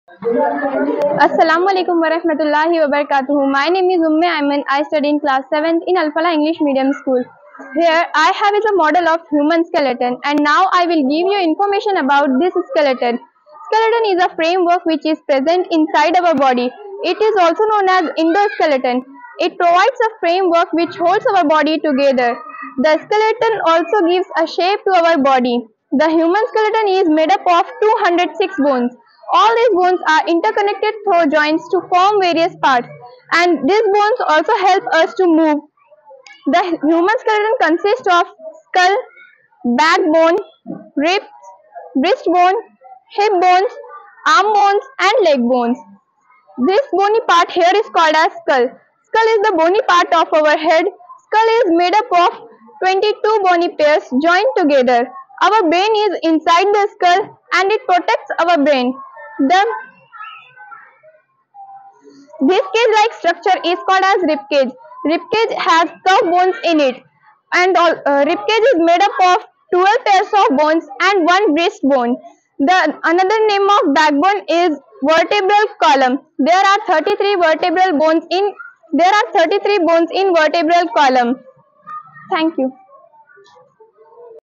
Human Skeleton by UMME AIMAN | CLASS 7TH | AL-FALAH ENGLISH MEDIUM SCHOOL
our student UMME AIMAN explained about the human skeleton and about the different bones